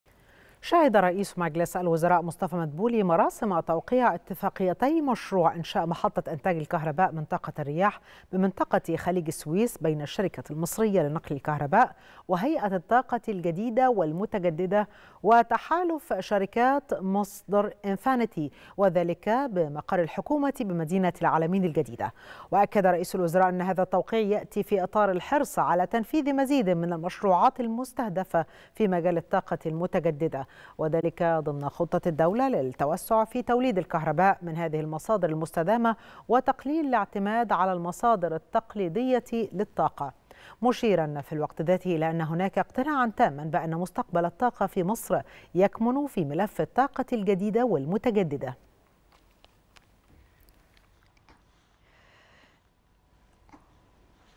مدبولي يشهد توقيع اتفاقيتي مشروع إنشاء محطة إنتاج الكهرباء من طاقة الرياح بخليج السويس
نشرة السادسة | مدبولي يشهد توقيع اتفاقيتي مشروع إنشاء محطة إنتاج الكهرباء من طاقة الرياح بخليج السويس